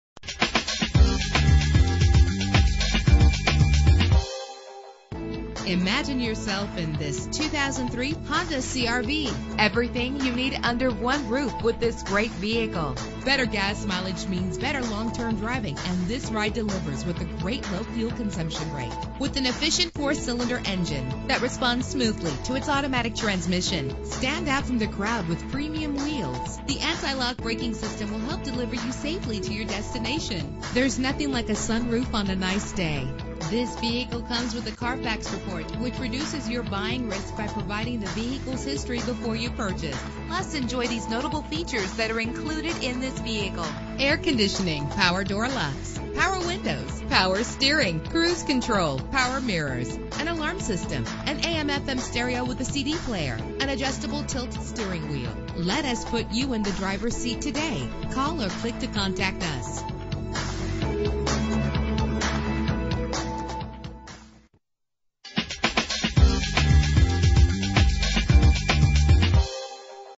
2003 Honda CR-V - CHESHIRE MA
Year: 2003
Make: Honda
Model: CR-V
Trim: EX
Engine: 2.4 liter inline 4 cylinder 16 valve
Transmission: 4-Speed Automatic
Color: Eternal Blue Pearl
Mileage: 91480
Address:
452 S State Rd